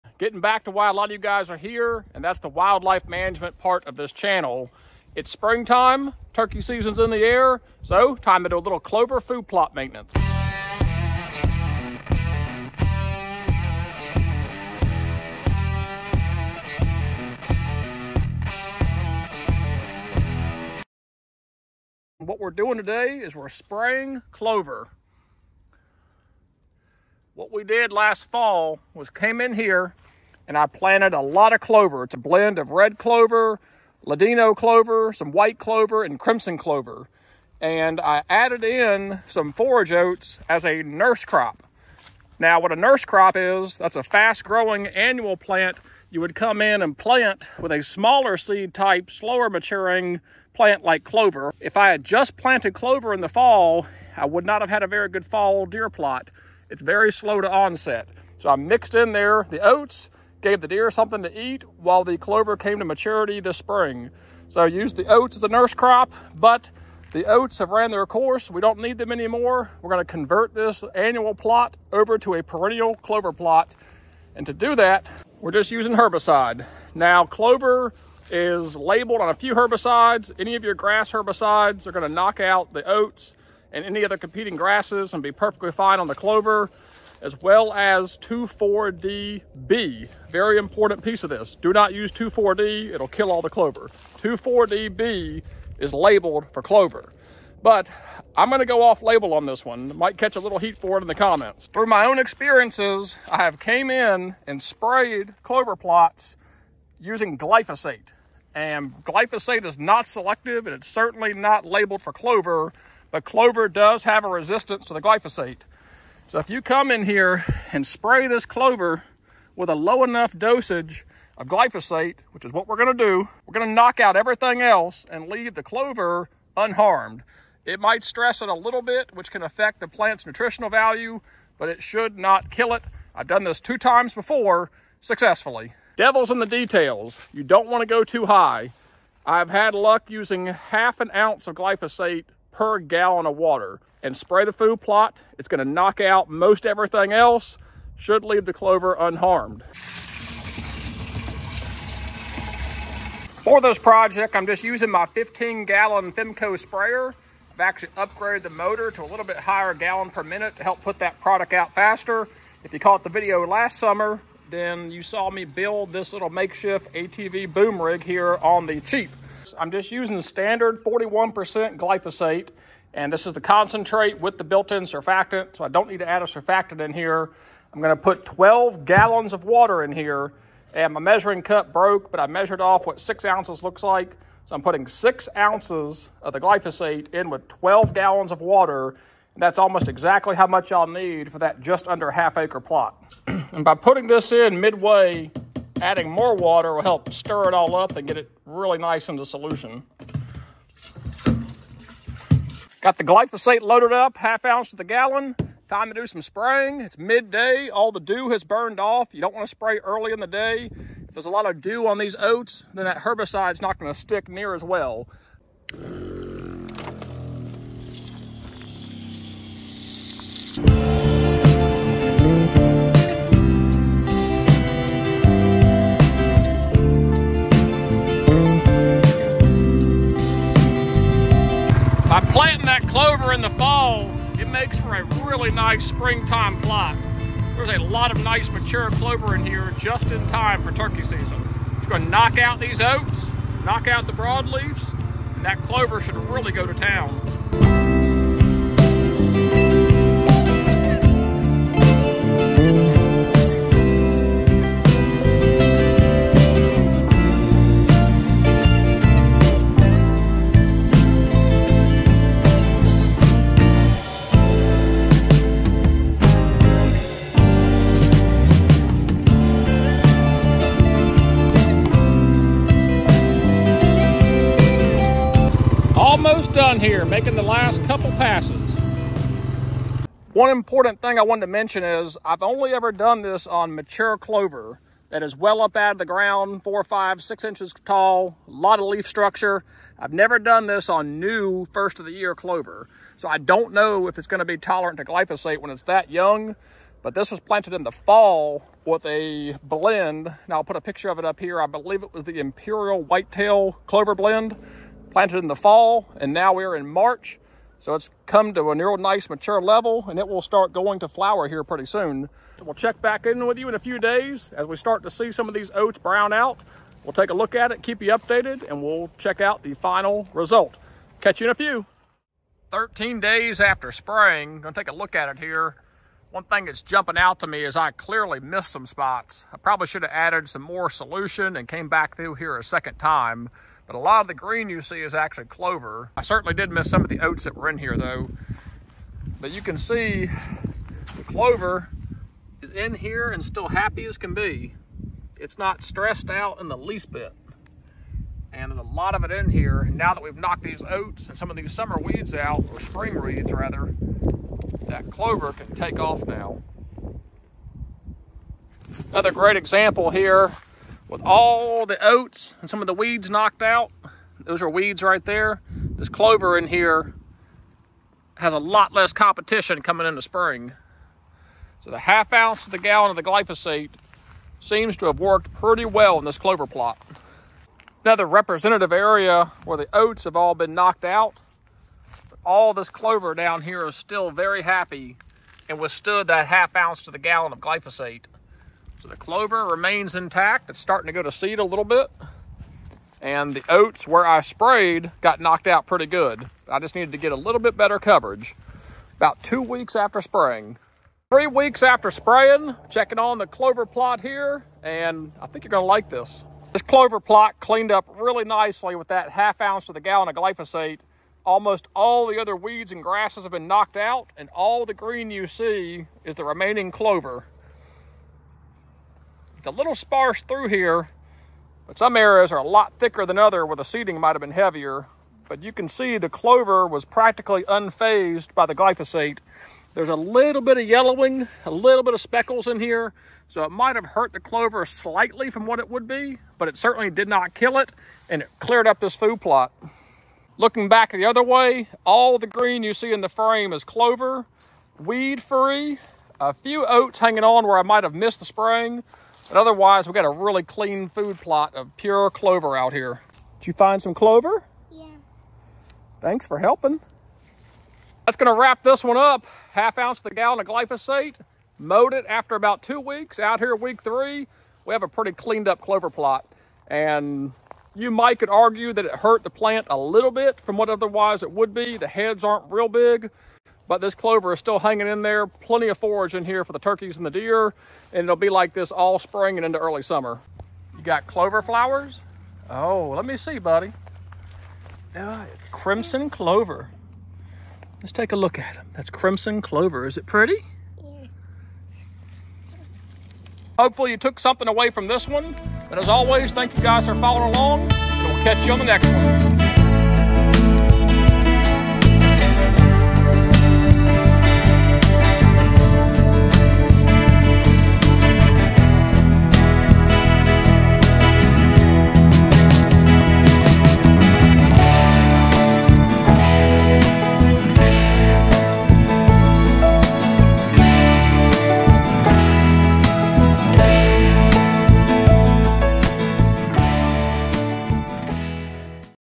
Will Glyphosate Kill a Clover Plot? Let's find out! | Spraying a Spring Clover Plot
Join us for some Spring Clover plot maintenance. Over spraying with Glyphosate for spring grass and weed control